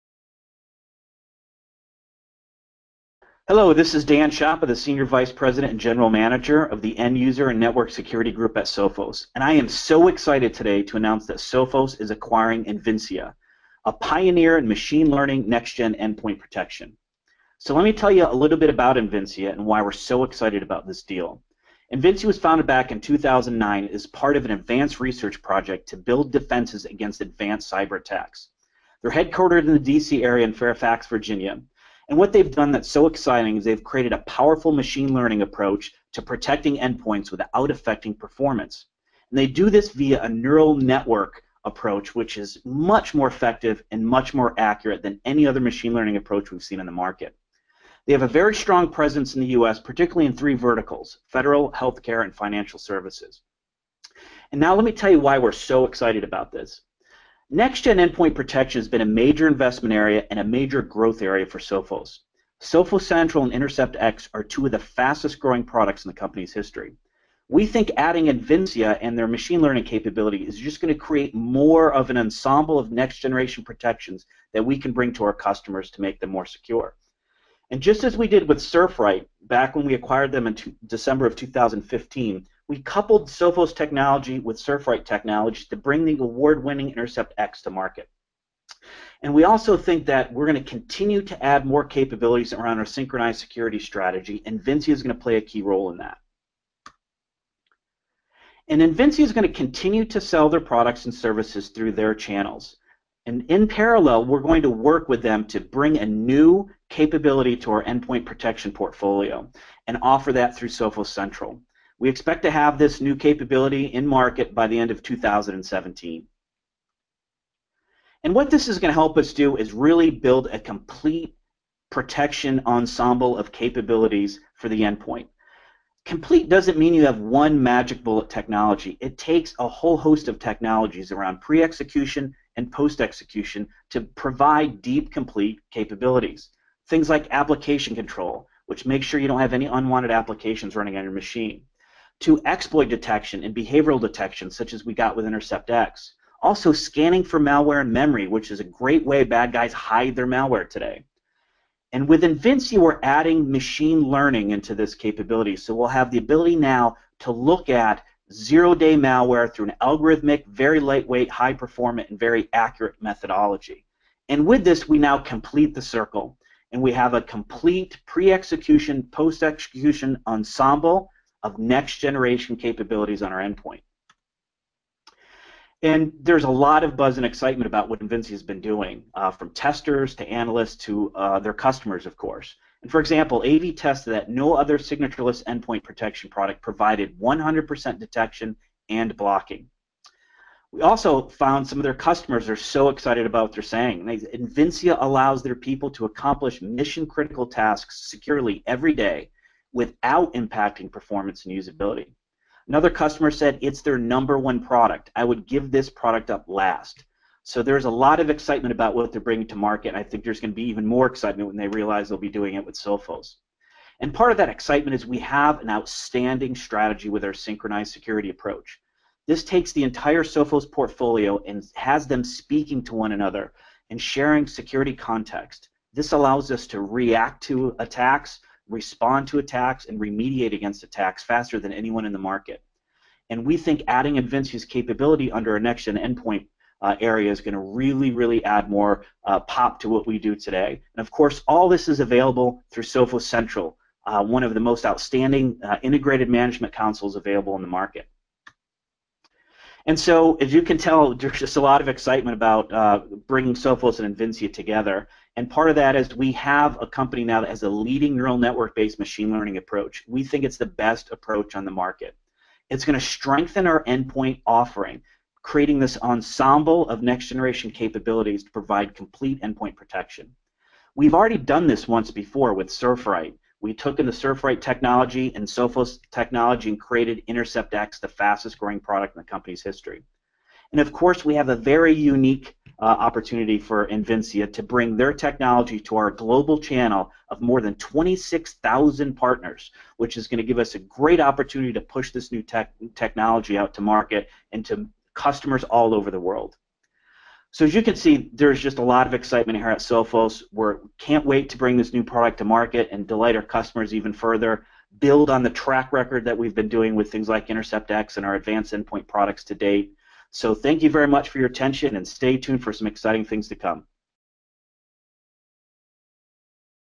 Sophos Welcomes Invincea
Invincea is a pioneer in machine learning next-gen endpoint protection and we're so excited to welcome this company and technology to the Sophos team. Dan Schiappa, Senior VP & GM of Enduser and Network Security Groups at Sophos talks about this exciting acquisition.